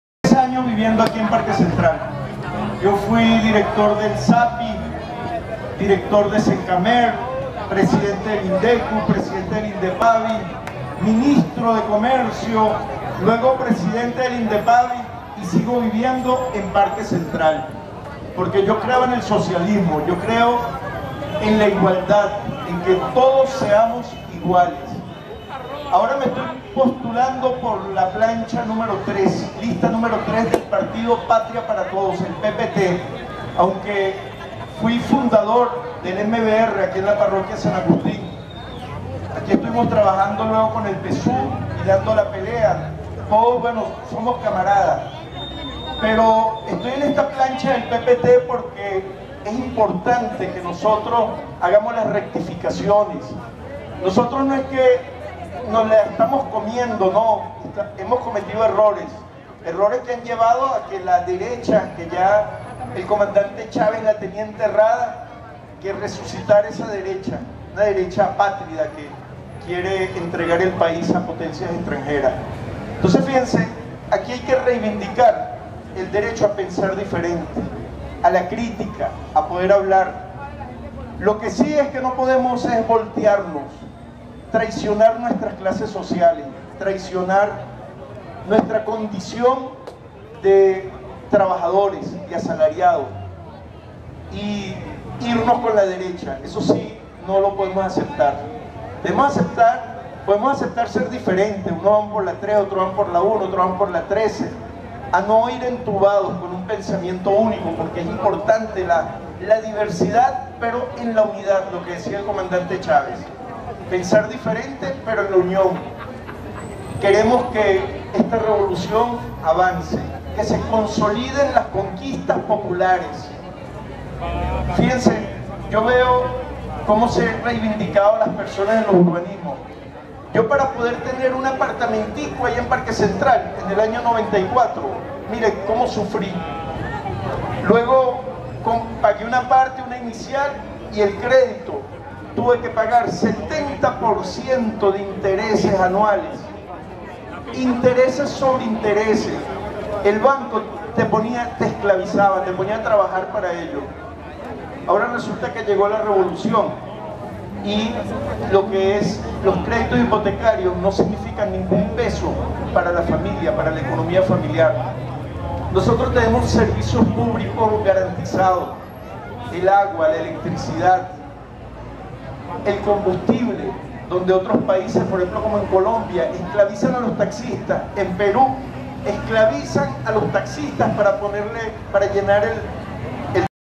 Candidato Eduardo Samán, en jornada de Simulacro Constituyente
El Trabuco Azul, lista #3 por Distrito Capital